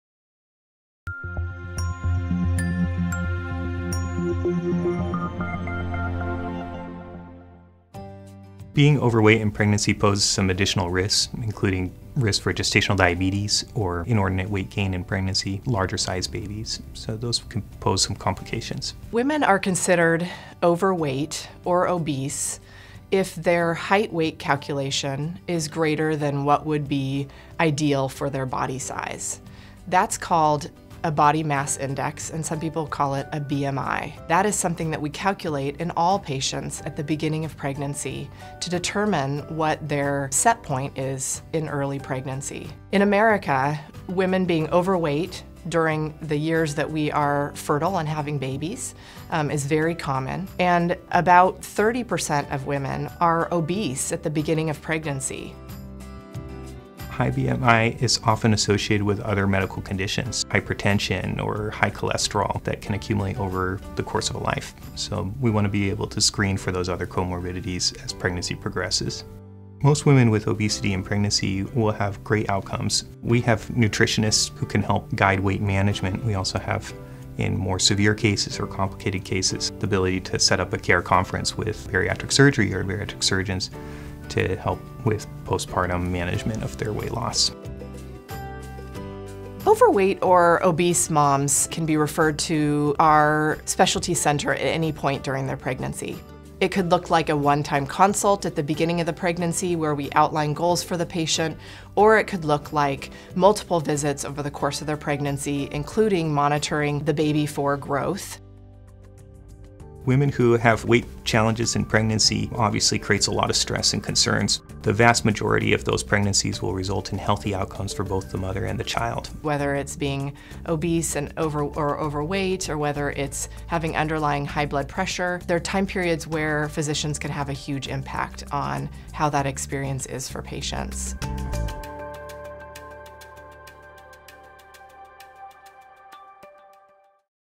High Risk Pregnancy: High BMI
Doctors of perinatology at the Swedish Maternal and Fetal Specialty Center explains the risks of being overweight during pregnancy.